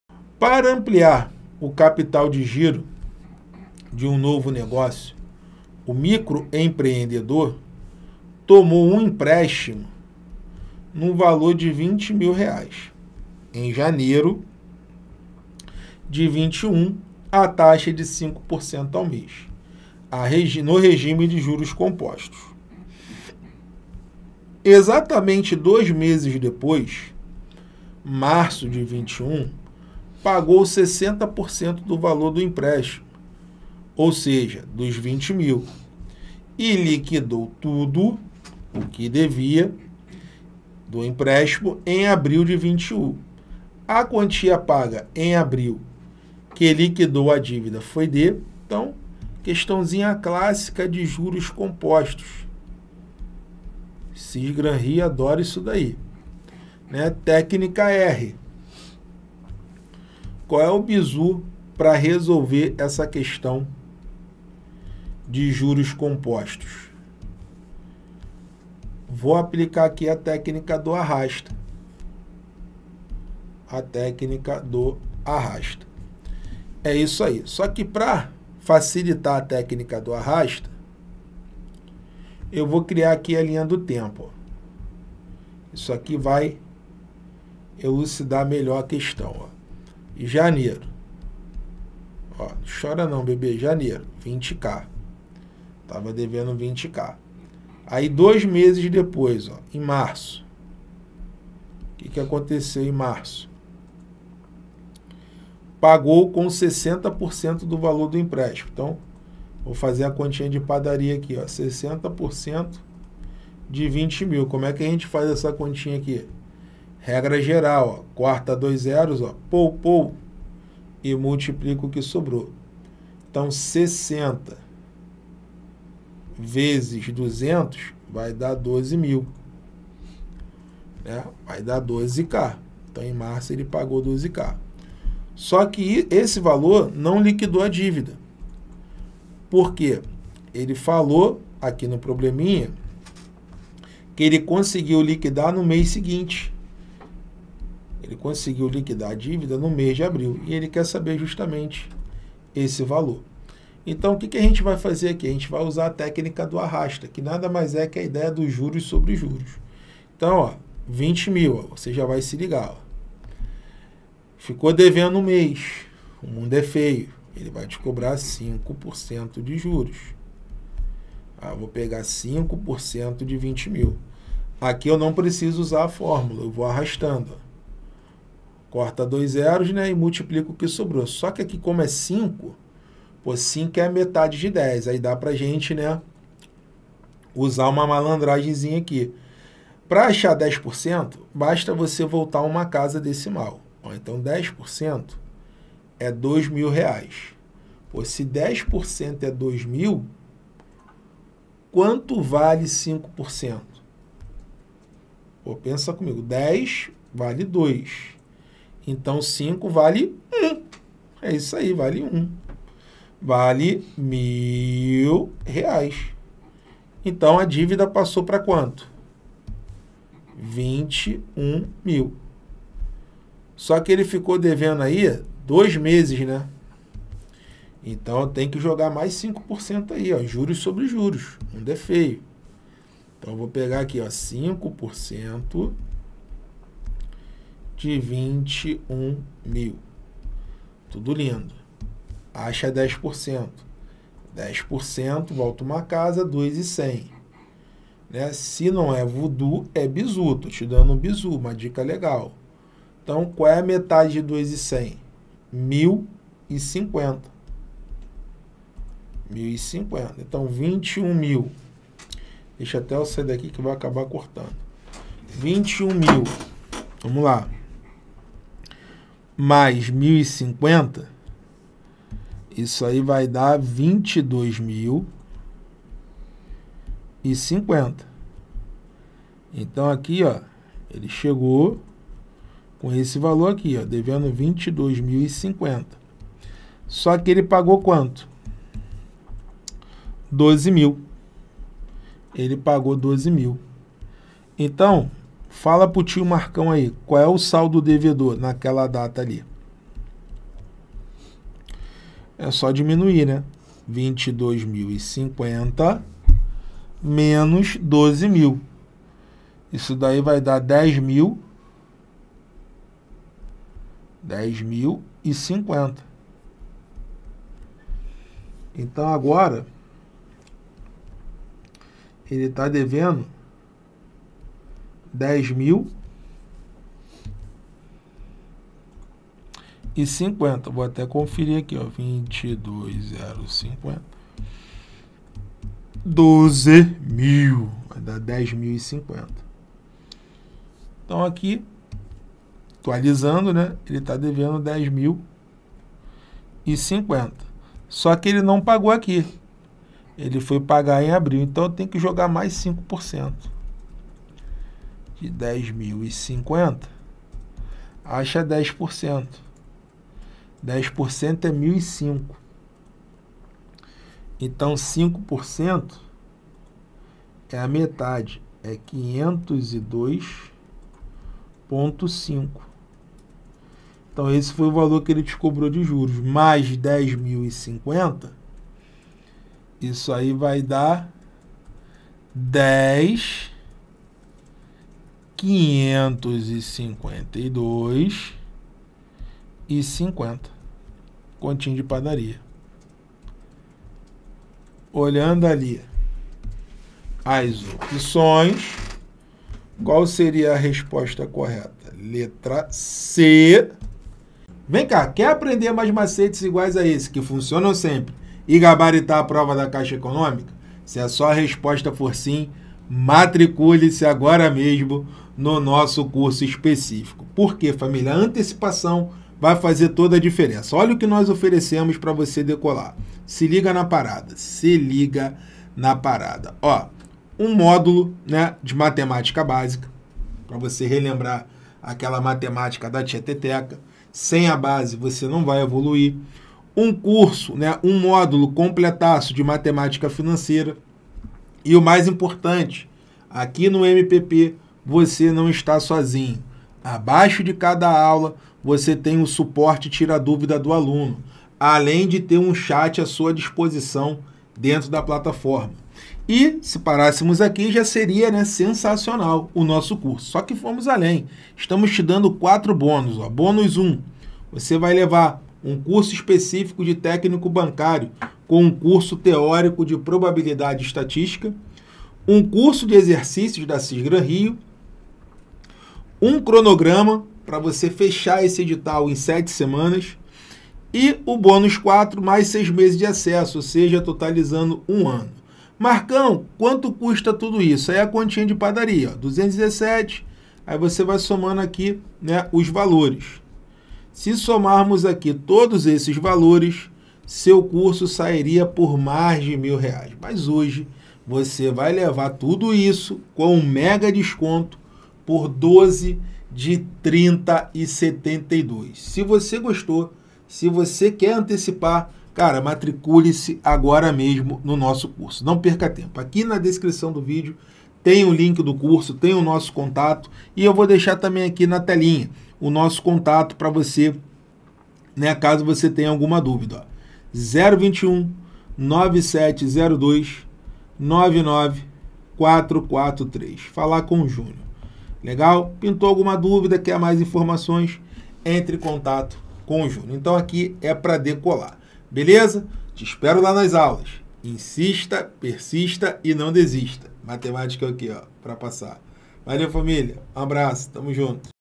Matemática Financeira para CEF | Juros compostos nunca foi tão fácil
Você está sofrendo com Matemática Financeira para Caixa Econômica Federal?

Então você precisa assistir essa aula onde nosso professor, Marcão, mostra Matemática Financeira com o Método MPP para o Concurso da Caixa de uma forma objetiva e simples.

Você terá um passo a passo para aprender Matemática Financeira e passar no concurso da Caixa e sair na frente de muitos concorrentes, no último concurso essa matéria foi a que mais reprovou e se você quer passar nesse concurso de agora precisa chegar pronto para Gabaritar.

Agora basta dar o play no vídeo e aprender Matemática Financeira para Caixa Economica Federal com o Método MPP